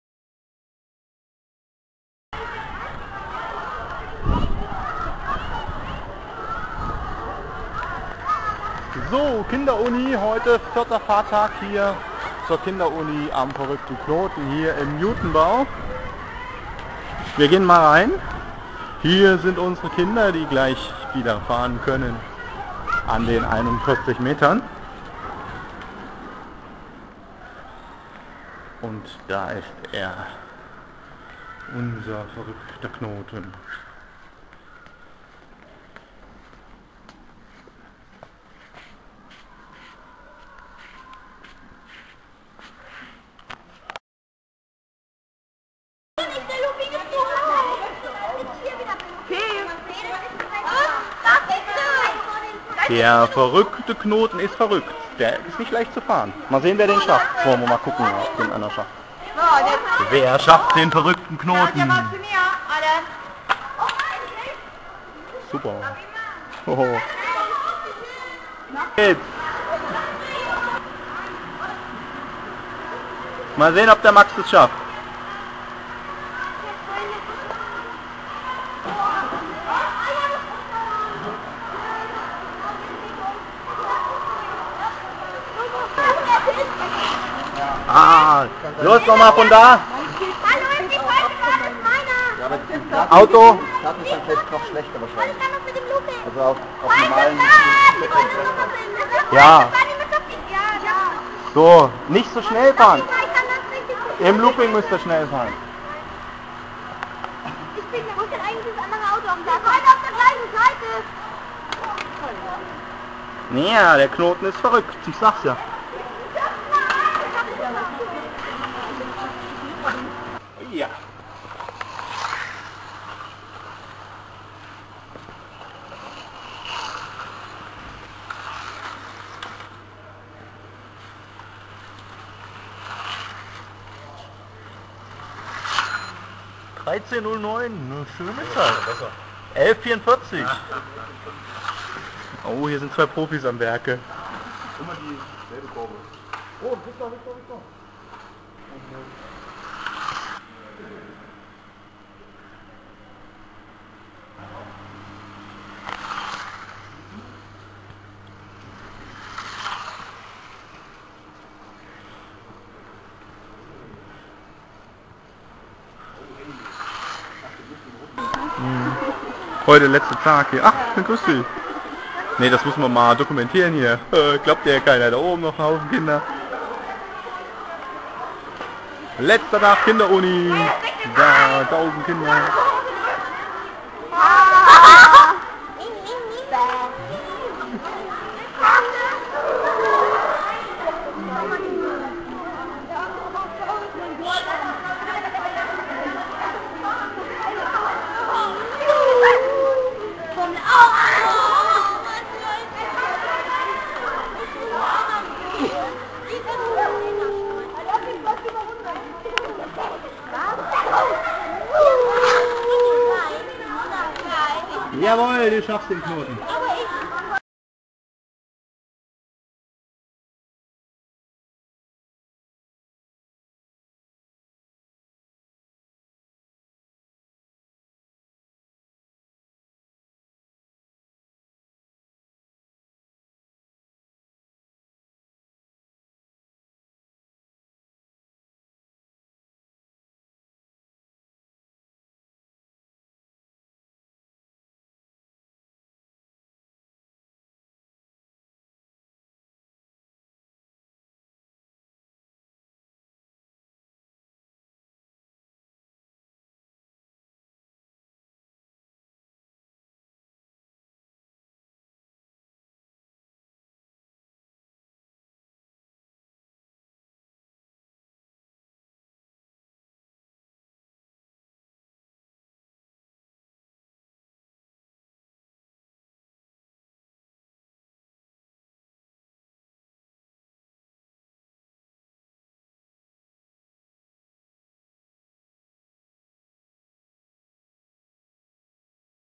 40 m lange Carrera Rennbahn @ Kinderuni Ilmenau 2009
Kai vom Carrera Club Thüringen baute vom 11. bis 25. November 2009 zu den fünf Kinderuni Tagen jeweils die 41 m lange Rennstrecke "Der verrückte Knoten".
Über 600 Kinder durften sich daran probieren.
Was für ein Spaß. Mit 12 Fahrzeugen wurden spannende Runden gefahren.
Die Mädchen fuhren genauso rasant wie die Jungs.
Einige sehr talentierte Nachwuchsfahrer wurden gesichtet.

Das Thema Elektromobilität wird sicher schon Alltag sein,
wenn die Schüler dann selber einmal studieren werden.

2010 gab es dann eine 50 m lange Rennstrecke in der Campus-Sporthalle die "Uni Runde".